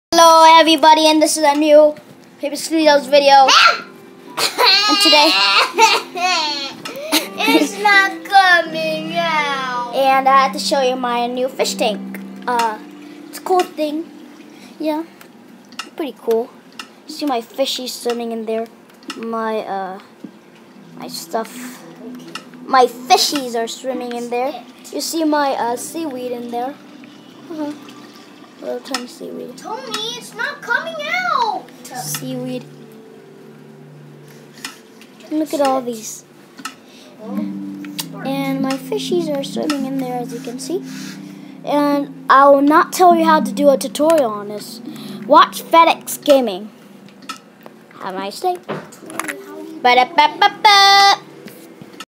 WORKING FISH TANK!!!!!!
OMG ITS A WORKING FISH TANK BUT IM JUST SHOWCASING THIS NO TUTORIAL SO. Go to fed x gaming